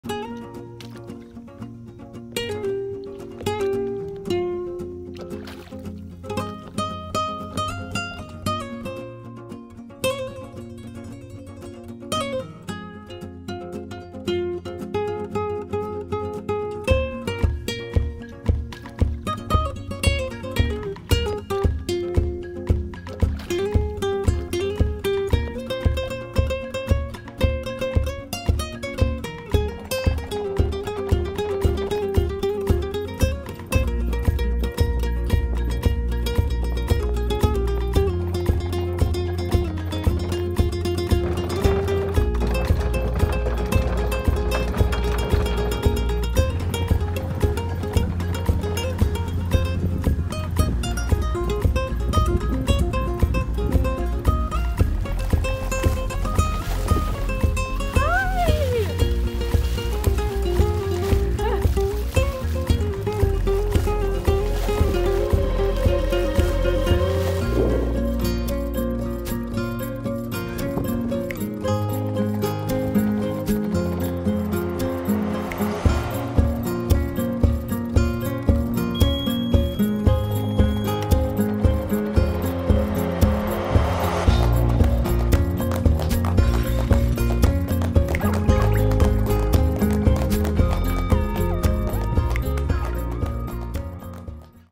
Sailing Moreton Bay // Ep. 1
A little video put together from the snippets I got on our 2 day sail from the Manly Marina Brisbane to Moreton Island, Lucinda Bay.